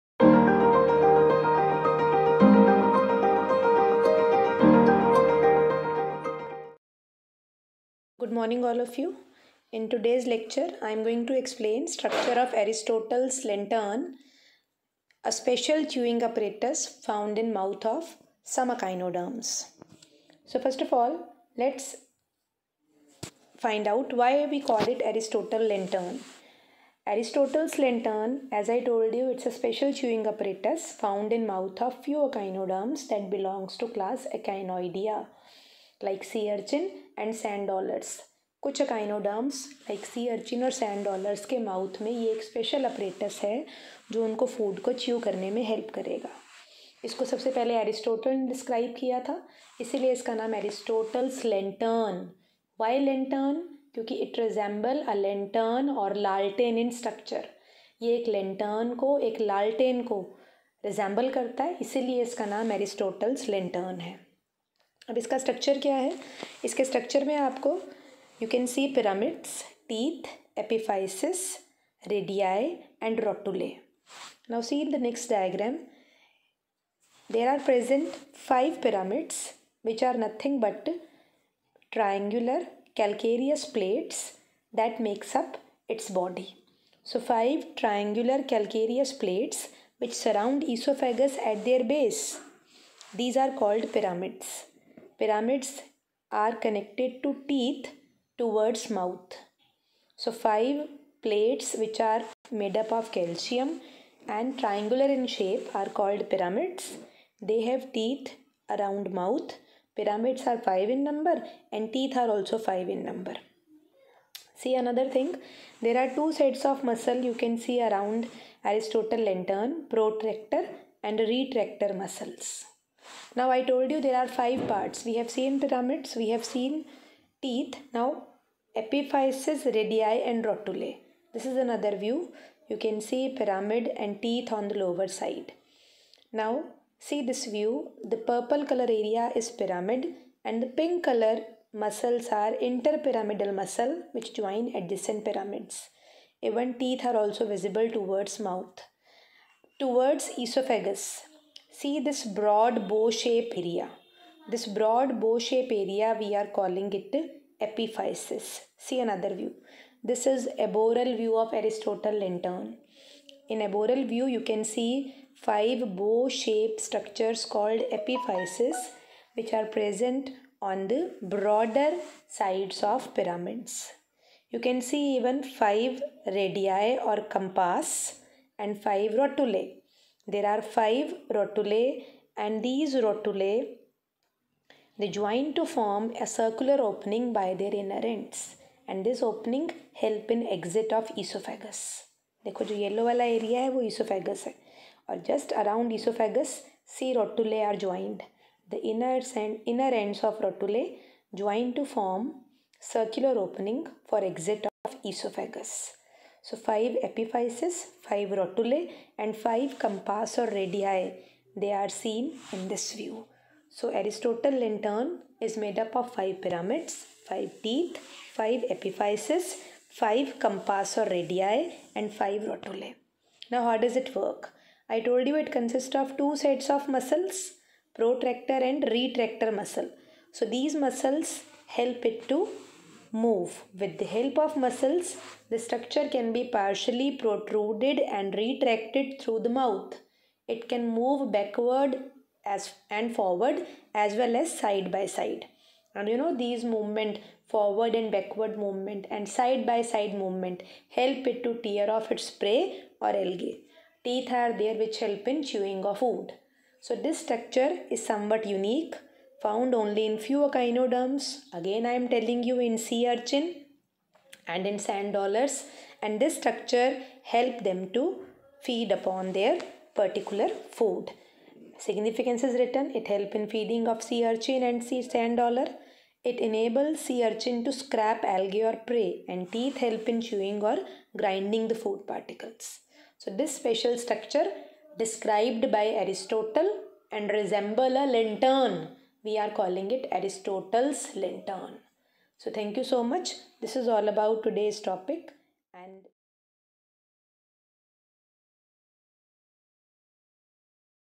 Aristotle's Lantern(Topic of B.Sc. 2nd Sem)
Description of structure and working of Aristotle's Lantern